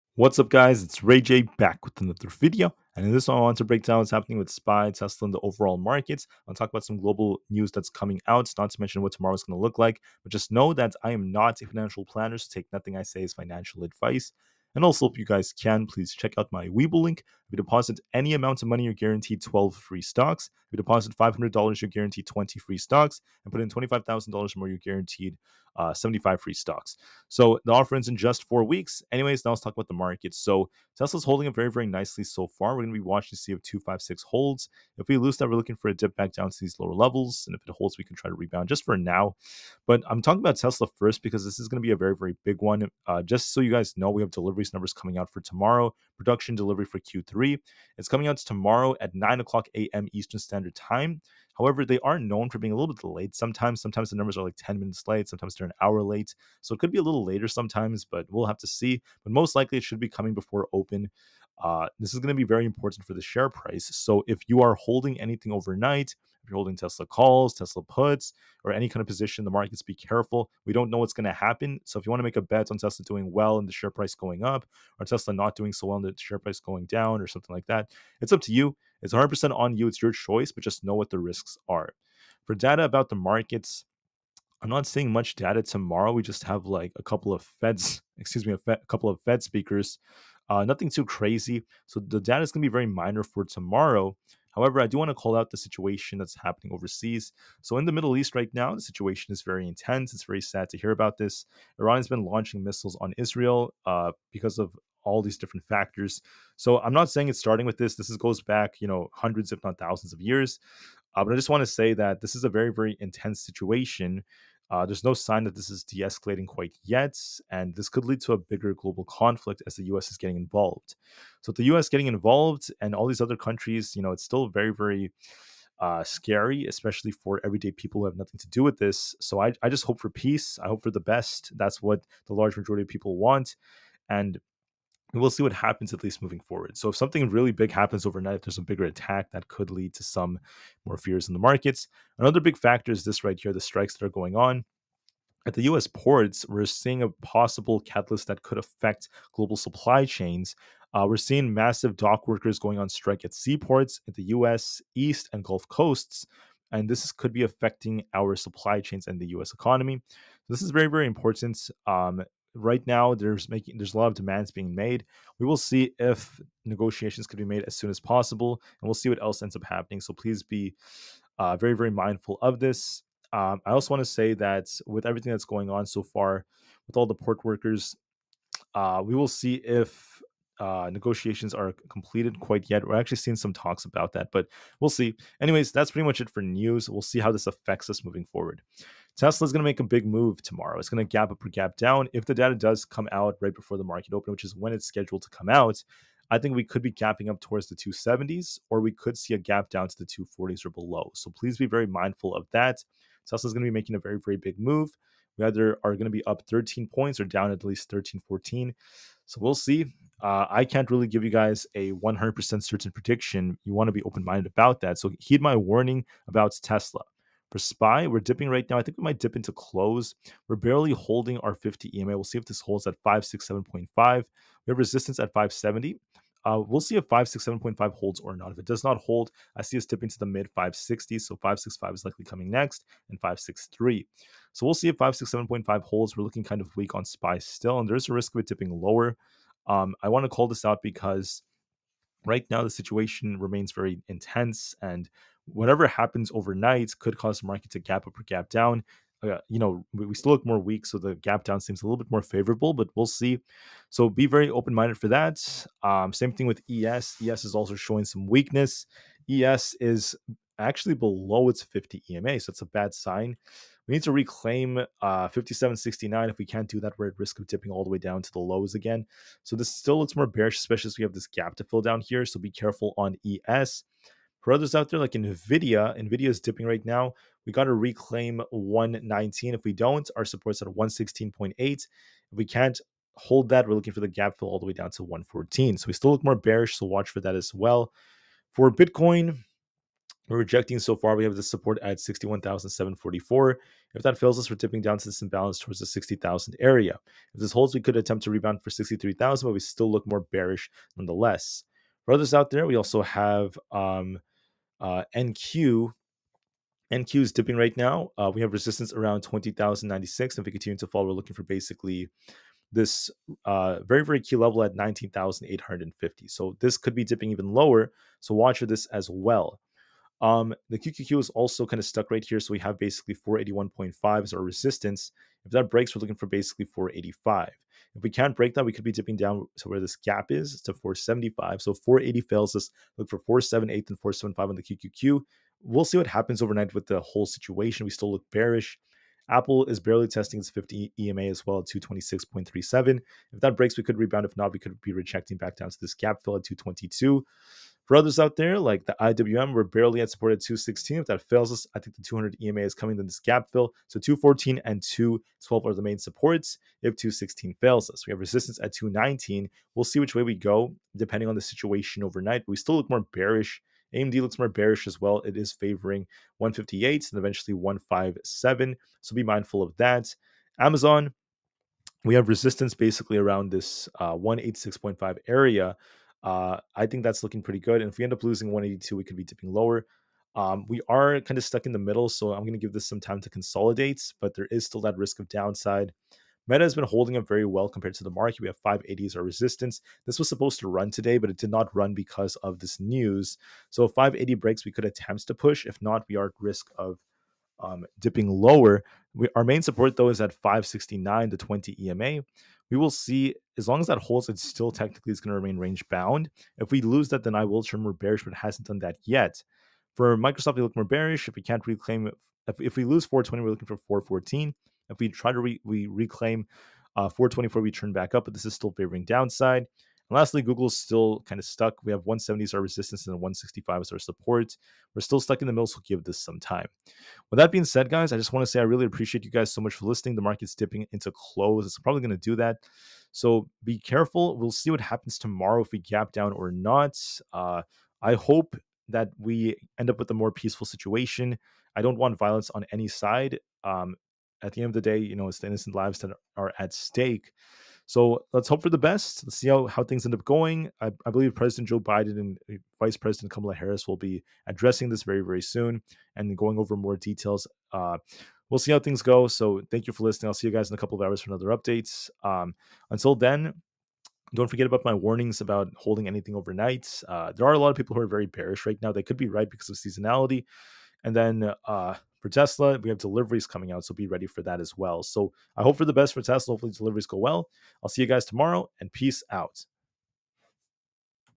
🔥 WARNING FOR TOMORROW!! SPY, ES, TSLA, NVDA, AAPL, COIN, META, GOOG, AMZN, & MSFT PREDICTIONS! 🚀
Hey everyone!

SPY and SPX had a green day on Monday as SPY closed at $573 (close to expectations), and it looked weaker than before. Despite that, SPY is still very strong for the long-term! In this SPY video, I will give you my SPX stock price prediction, while mentioning SPY and stock market news; regardless of SPX and SPY's short terms, their long-terms will be bright with massive growth potential. SPY to the moon! SPY is the future!